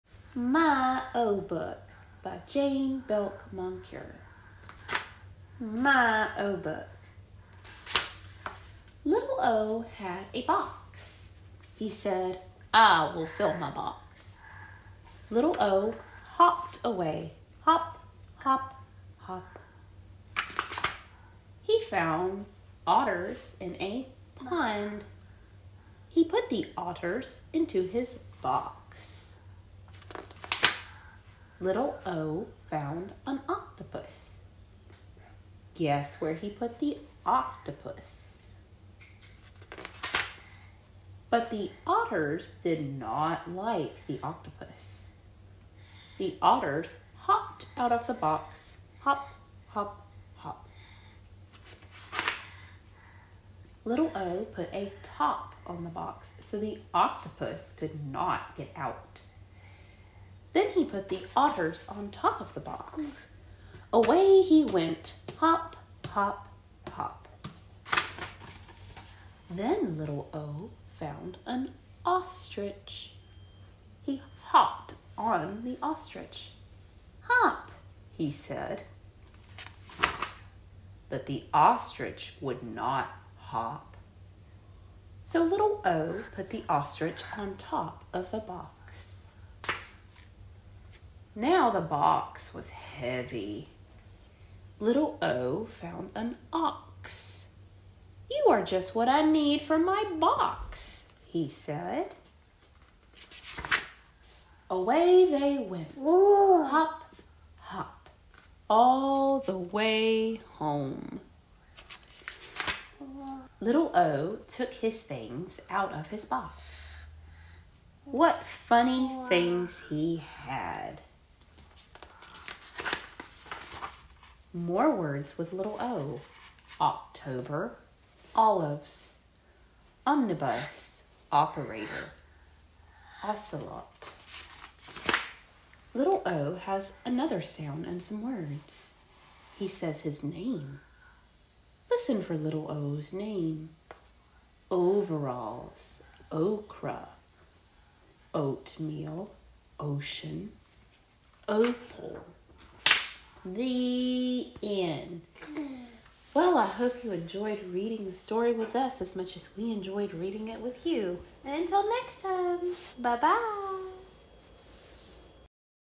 My “o” book by Jane Belk Moncure - Read Aloud and Read Along
We hope you enjoy reading along with us as much as we enjoy reading along with you! We are really enjoying this kid book series and are so happy that you have come to join us for storytime!